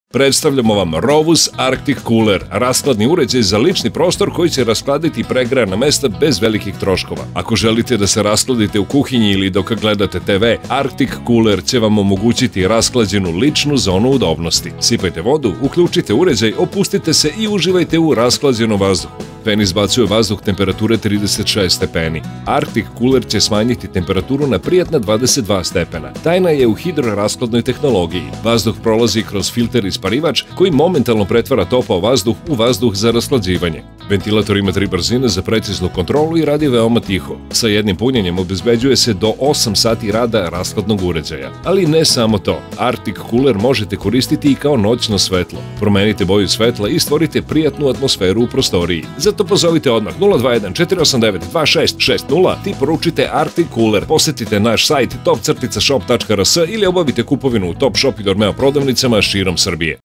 Rovus Arctic Cooler 3 u 1 rashladni uređaj
Rovus Arctic Cooler je inovativan, prenosiv 3u1 rashladni uređaj koji hladi, osvežava i vlaži vazduh u prostoriji.  Poručite ovde